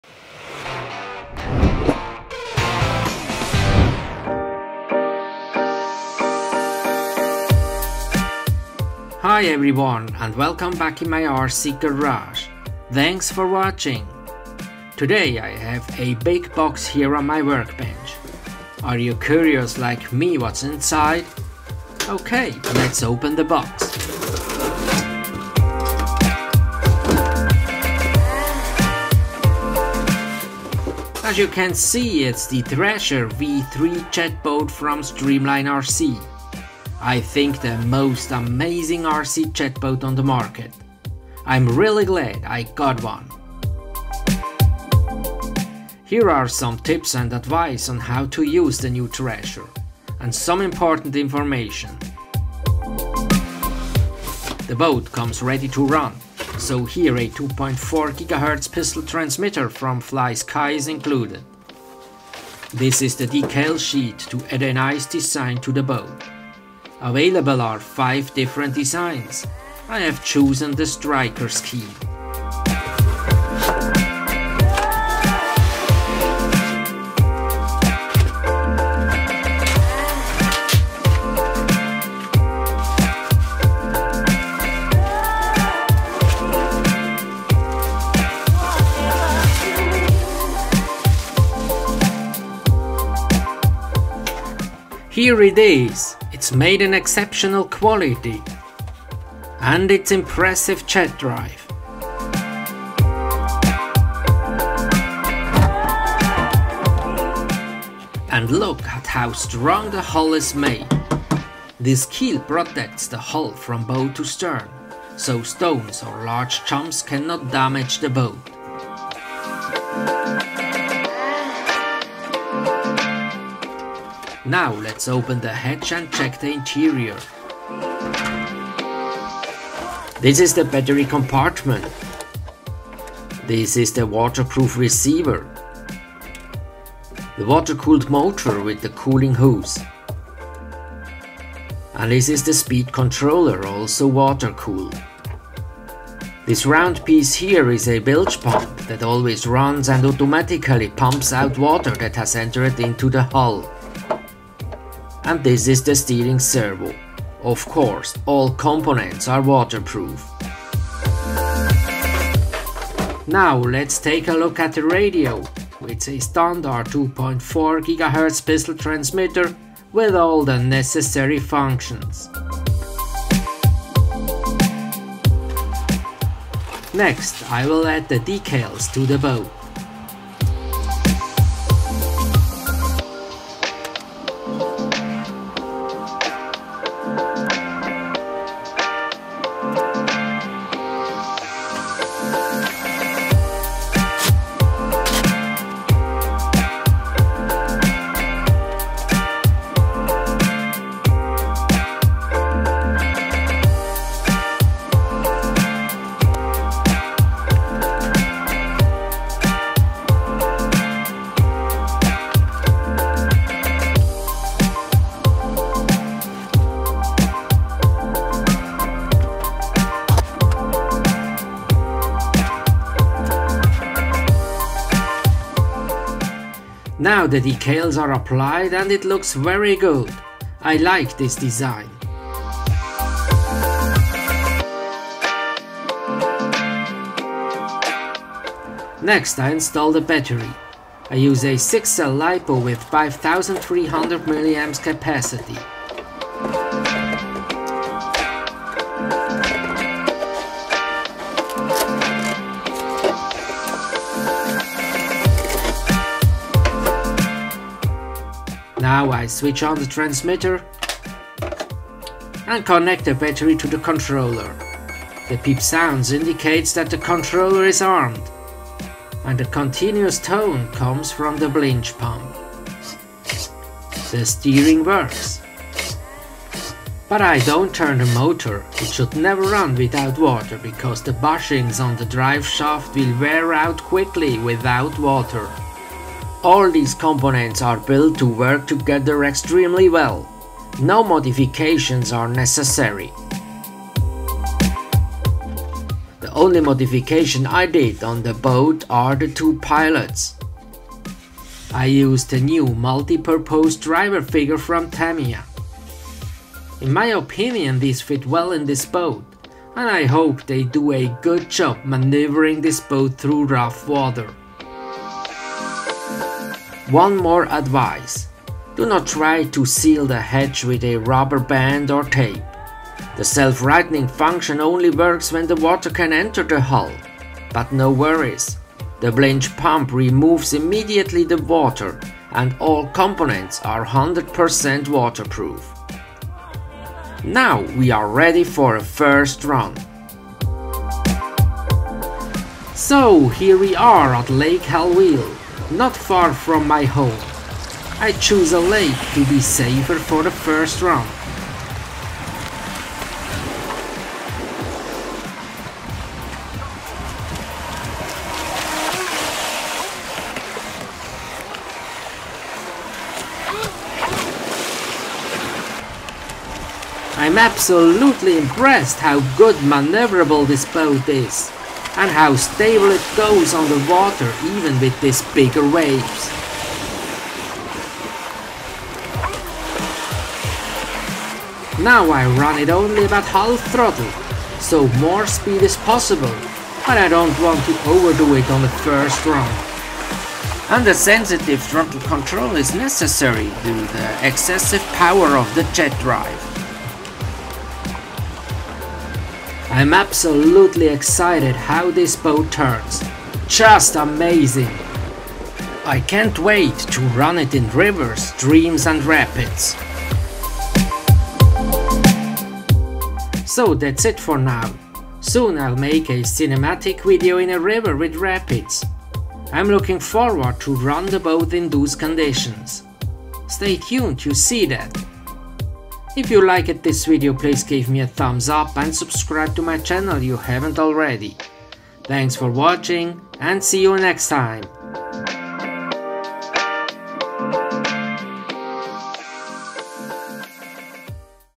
Thrasher V3 Jetboat - Unboxing and Review
This Jet Boat from Streamline RC is absolutely amazing. Extremely well designed and so much fun to drive.